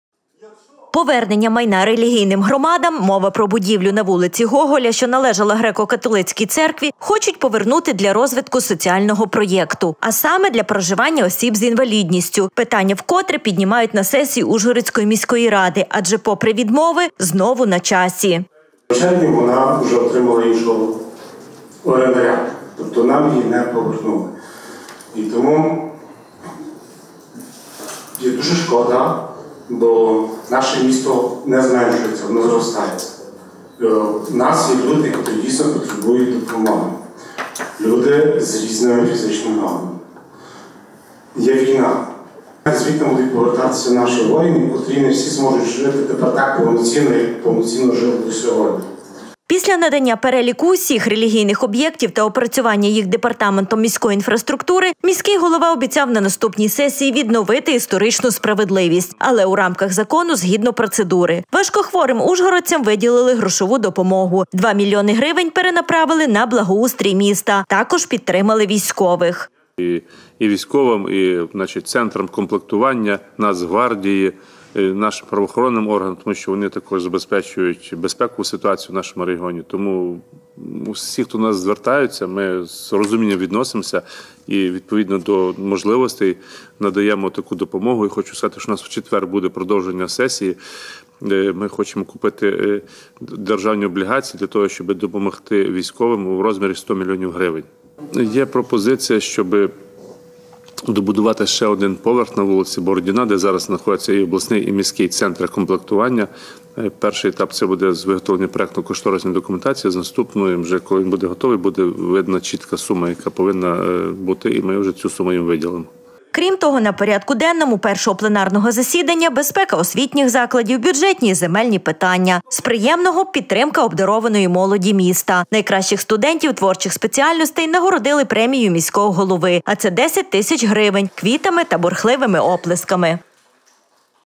Сесійні рішення. Життя міста
Матеріальна підтримка військових формувань та територіального центру комплектування, важкохворих ужгородців, безпека освітніх закладів, бюджетні питання, благоустрій міста та інші не менш важливі рішення зібрали депутатів Ужгородської міської ради на перше пленарне засідання чергової ХХІІ сесії. На порядку денному понад пів сотні нагальних питань для забезпечення життєдіяльності обласного центру. Повернення майна релігійним громадам – мова про будівлю  на вулиці  Гоголя, що належала греко-католицькій церкві – хочуть повернути для розвитку соціального проєкту. А саме для проживання осіб з інвалідністю. Питання вкотре піднімають на сесії Ужгородської міської ради, адже попри відмови - знову на часі. Після надання переліку усіх релігійних об’єктів та опрацювання їх департаментом міської інфраструктури, міський голова обіцяв на наступній сесії відновити історичну справедливість, але у рамках закону згідно процедури. Важкохворим ужгородцям виділили грошову допомогу. Два мільйони гривень перенаправили на благоустрій міста. Також підтримали  військових. Крім того, на порядку денному першого пленарного засідання: безпека освітніх закладів, бюджетні і земельні питання. З приємного – підтримка обдарованої молоді міста. Найкращих студентів творчих спеціальностей нагородили  премією міського голови- а це 10 тисяч гривень, квітами та бурхливими оплесками.